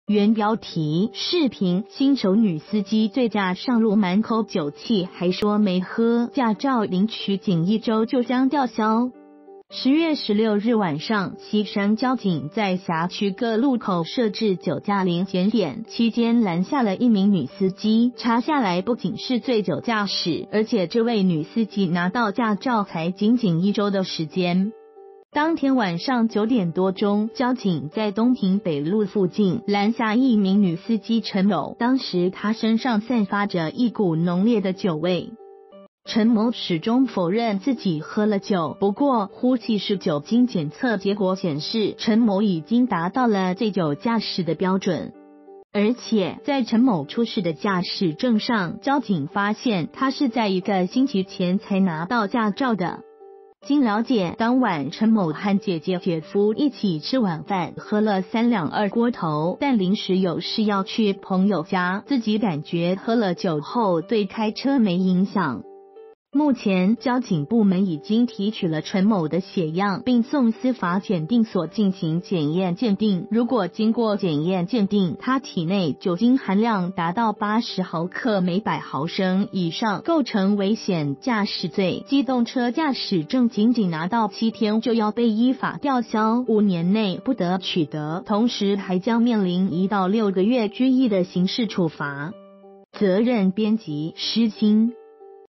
女司机醉驾上路满口酒气说没喝 驾照领取仅一周
原标题：视频|新手女司机醉驾上路满口酒气还说没喝 驾照领取仅一周就将吊销

發表評論評論如下。